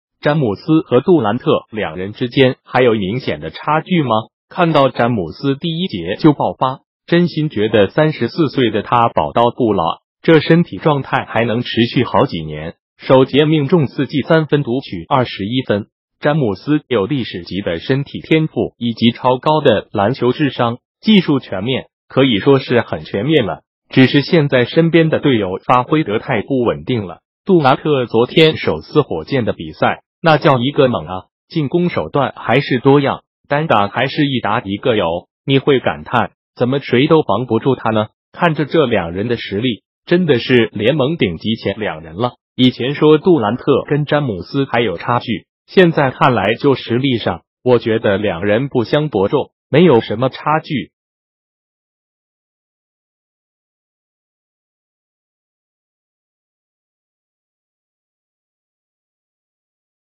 詹姆斯和杜蘭特兩人之間還有明顯的差距嗎
看到詹姆斯第一節就爆發，真心覺得34歲的他寶刀不老，這身體狀態還能持續好幾年！首節命中4記三分獨取21分！